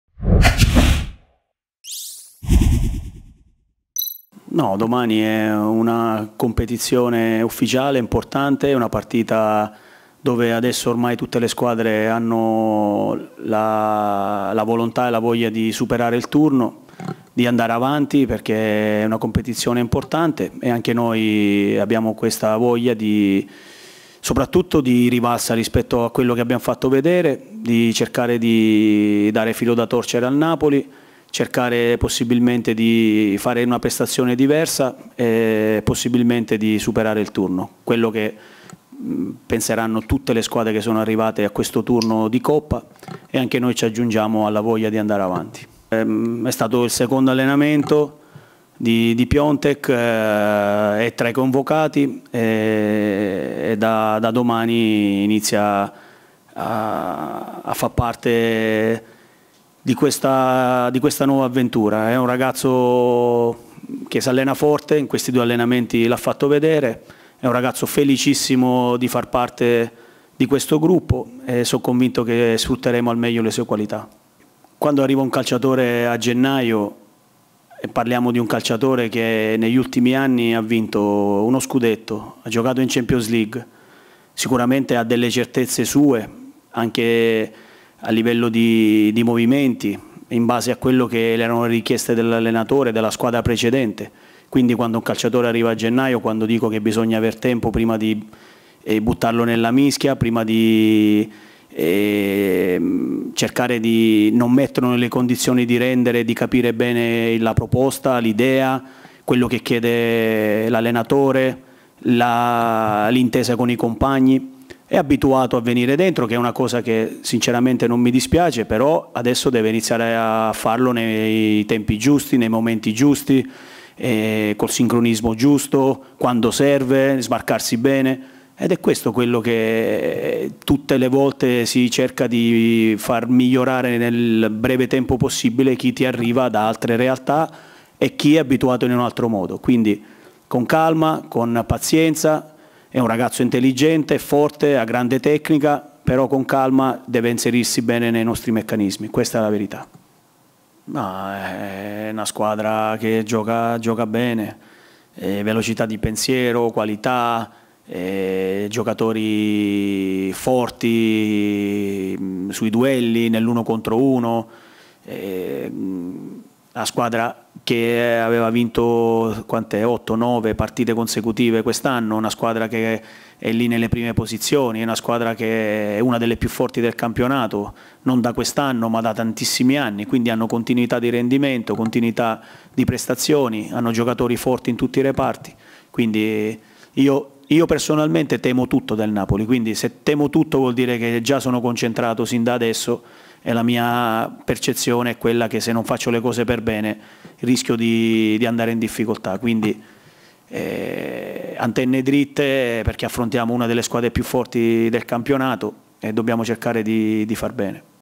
Conferenza stampa Italiano pre Napoli-Fiorentina di Coppa Italia: "Del Napoli temo tutto"
Il tecnico del Fiorentina Vincenzo Italiano è intervenuto in conferenza stampa alla vigilia di Napoli-Fiorentina.